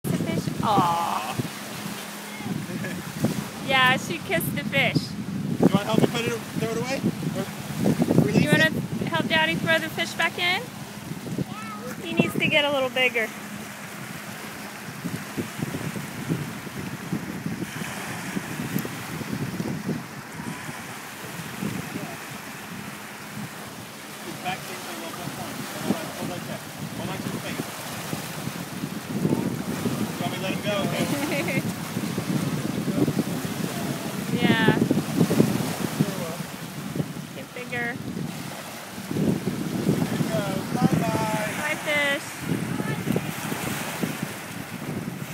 Fishy Kiss
Calla kisses a fish!